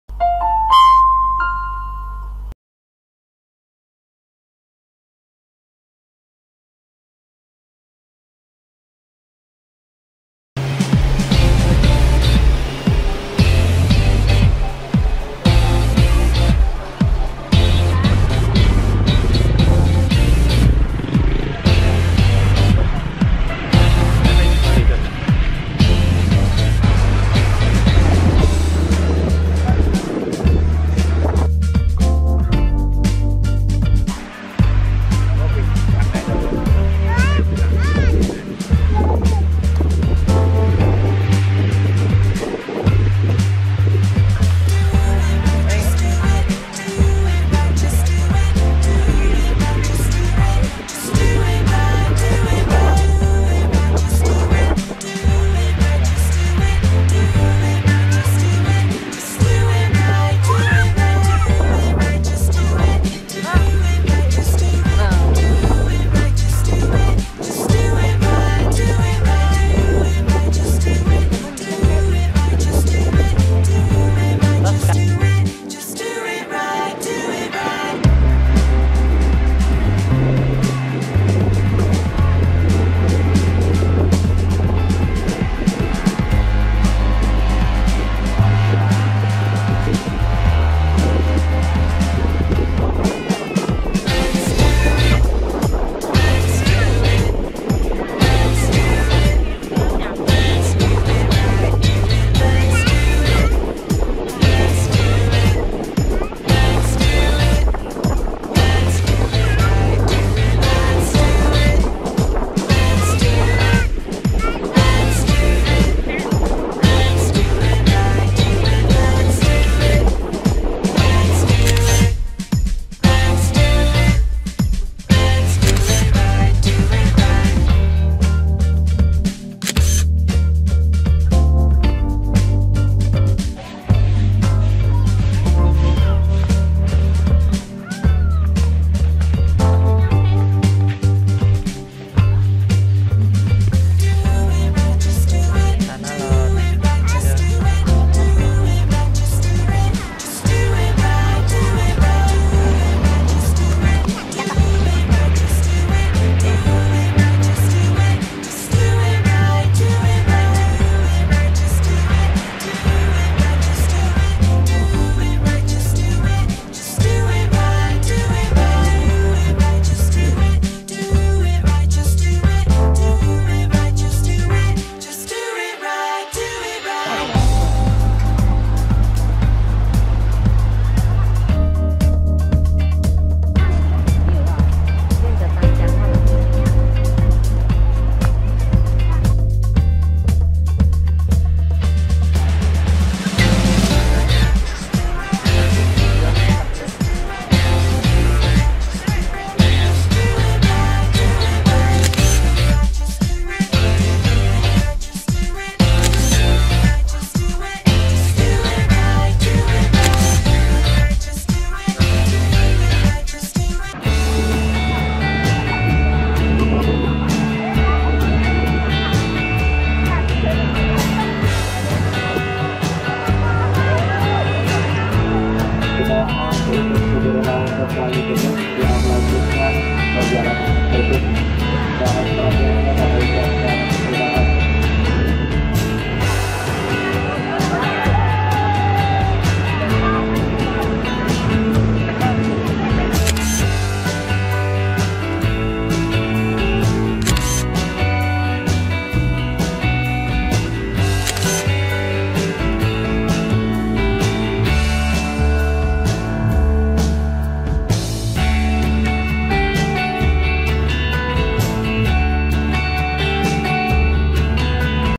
TANAH LOT - KUTA BEACH - BALI
Kita sudah sampai di Bali nih...
Video kali ini kita ke Kuta Beach dan Tanah Lot
Yukk... langsung aja di tonton...